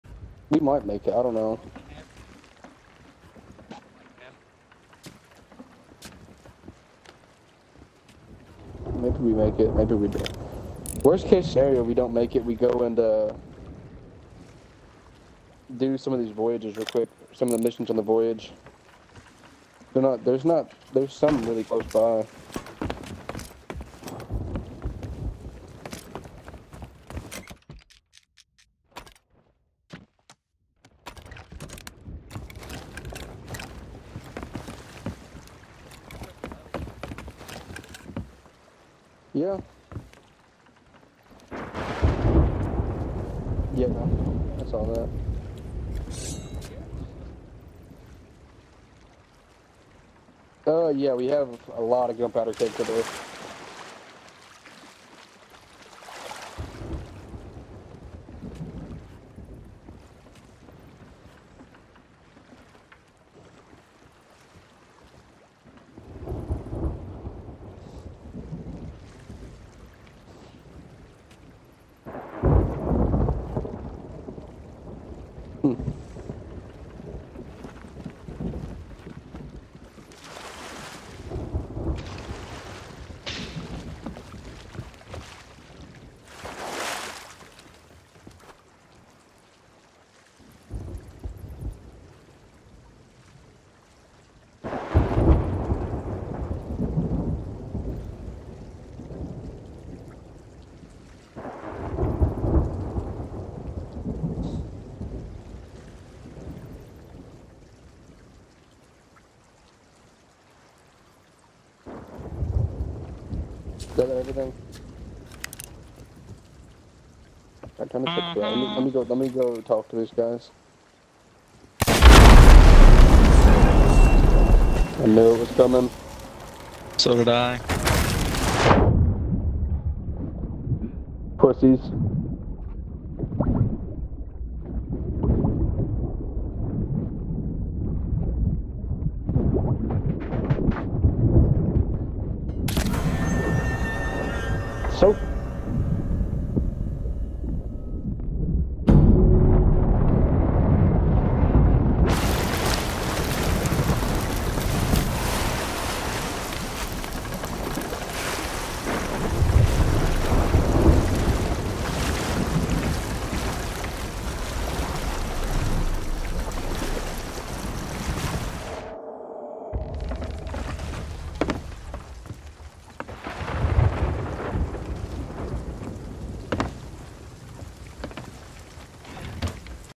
Part Two: The ish goes down....
Part Two: I'm taking the Funny Chest, and the Loot. This dude decided to split the loot 90/10 w him getting the Chests and giving us an Athena crate, and 1 other piece. He said we could have the kegs as payment (Flag). I wad like, hell 👎. toy know what, I'm good. you can have them. and like a Dingus, they drag them up there! My crew even helps them carry the kegs up there!!! RoFlmfa smh!

So I'm just waiting for Dingus #2 to get close to the blast zone so hopefully it gets em both and I don't have to Hunt them down in the water. Took long enough but the payout was worth it.